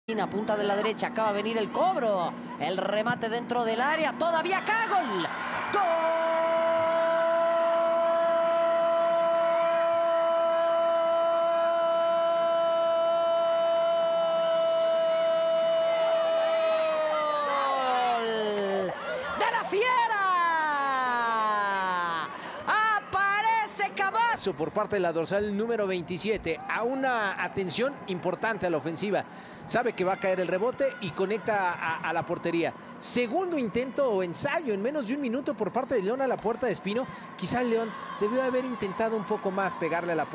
Gol de S. Camacho | Chivas 2 - 1 León | LigaBBVAMXFemenil | GritaMéxicoA21 J3
¡¡ GOLAZOOOOOOO DE SANDRA CAMACHO !!

⏱| 87´ Chivas Femenil  2️⃣ - 1️⃣ Club León Femenil